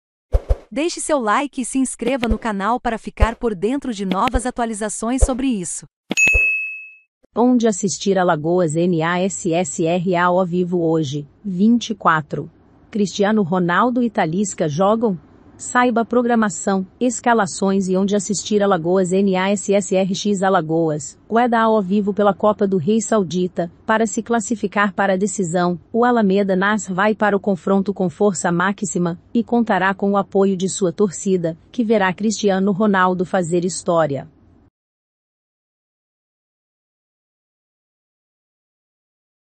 ONDE ASSISTIR AL NASSR AO VIVO HOJE (24): CRISTIANO RONALDO E TALISCA JOGAM? SAIBA...
ONDE ASSISTIR AL NASSR AO VIVO HOJE (24): CRISTIANO RONALDO E TALISCA jogam? Saiba programação. escalações e onde assistir AL NASSR X AL WEHDA AO VIVO pela Copa do Rei Saudita

Al Nassr quer se classificar para a final. vem para o jogo com força máxima e terá o apoio de sua torcida que vai ver Cristiano Ronaldo fazer história.